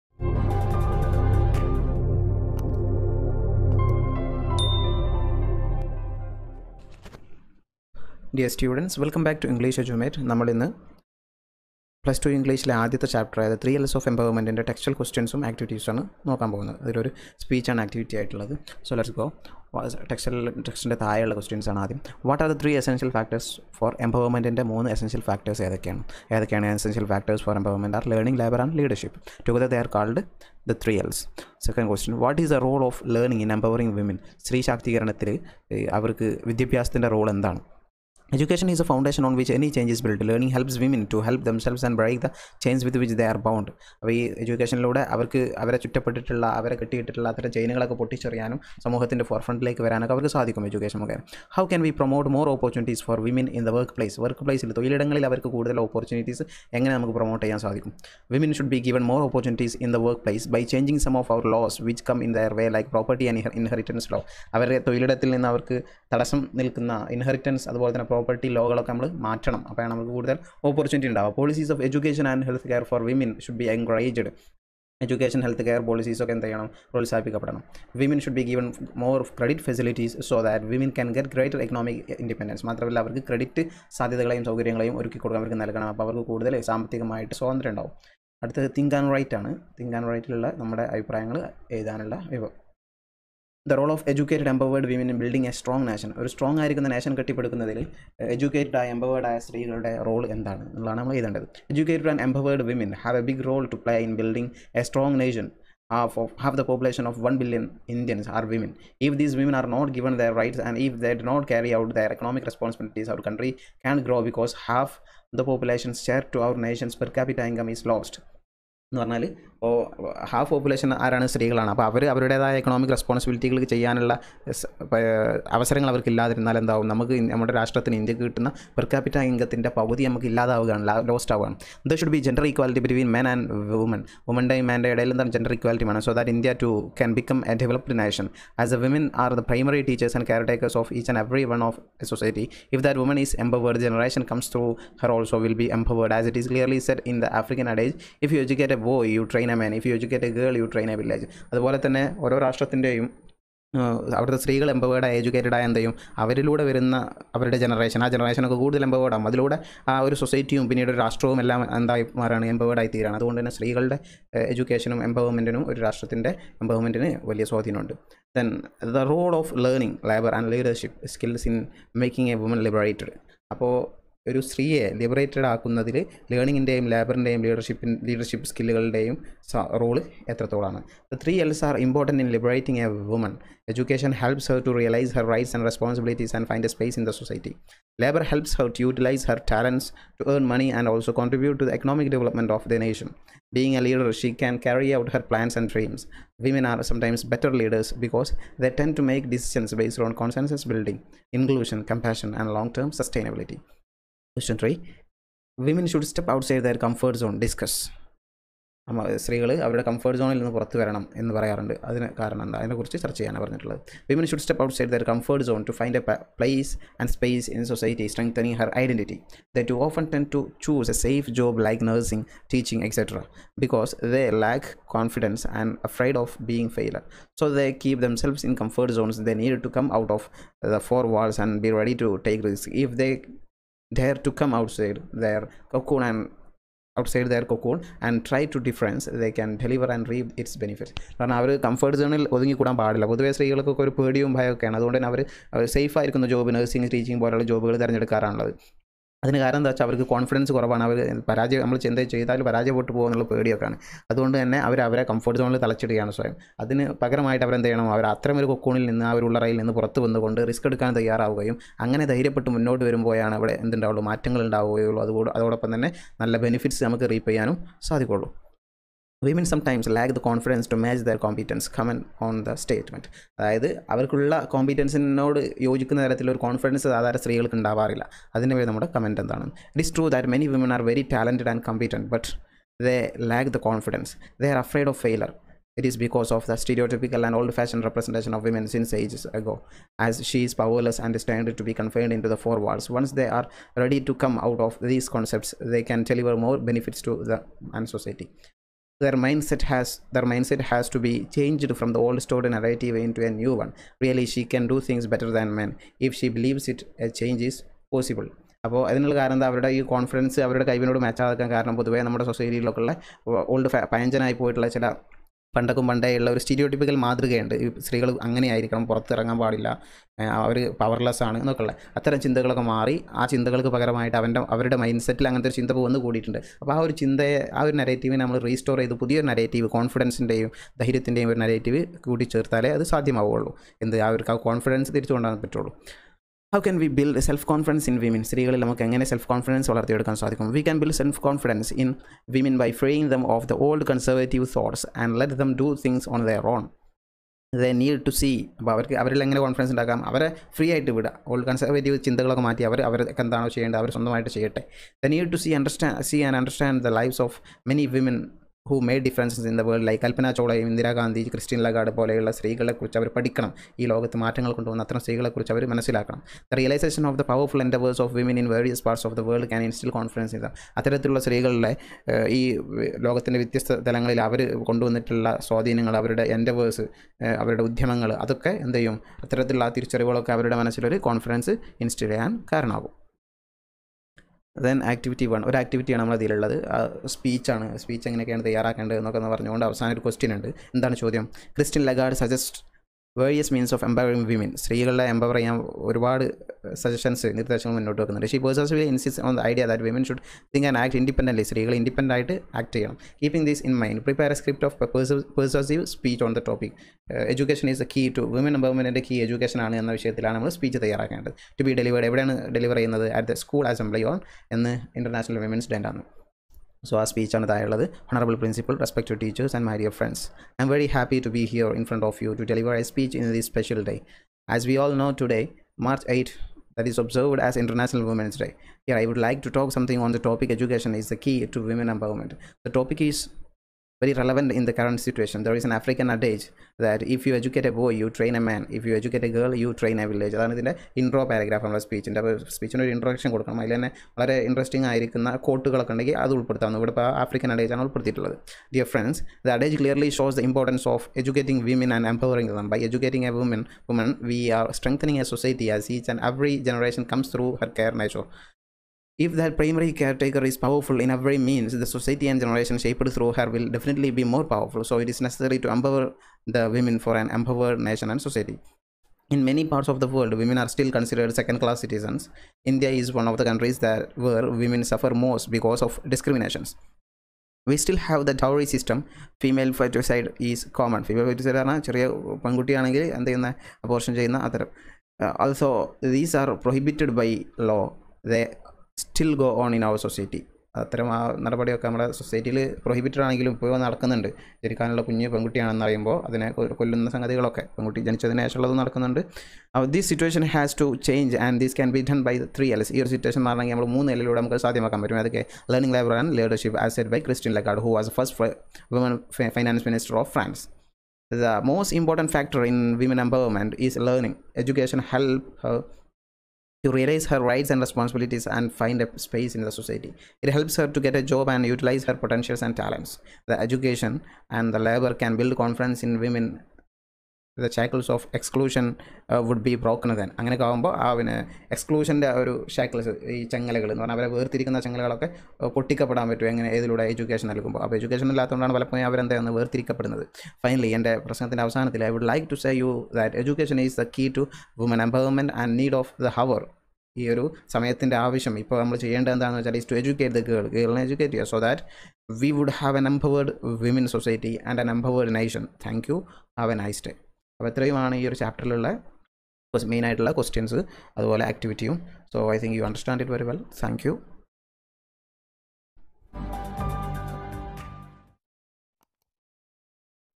Textual questions and activities/ 3 Ls of empowerment/ Plus two English
it is a video tutorial of the textual questions and activities of the lesson 3 Ls of Empowerment for Plus Two students.